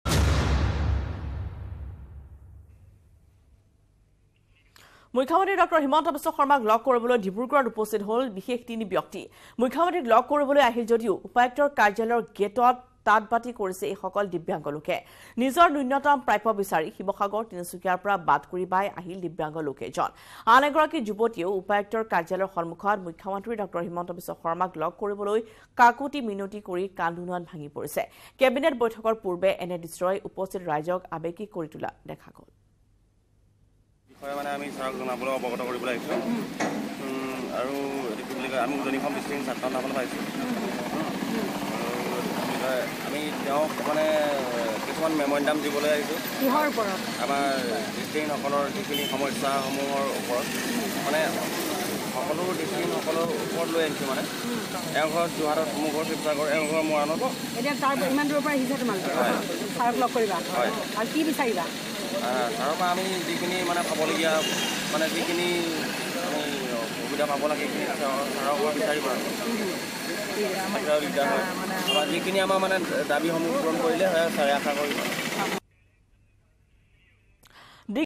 Special Visitors For CM Himanta Biswa Sarma | CM ক লগ কৰিবলৈ Dibrugarh ত উপস্থিত বিশেষ তিনি ব্যক্তি
Special Visitors For CM Himanta Biswa Sarma | মুখ্যমন্ত্ৰী ড০ হিমন্ত বিশ্ব শৰ্মাক লগ কৰিবলৈ ডিব্ৰুগড়ত উপস্থিত বিশেষ তিনি ব্যক্তি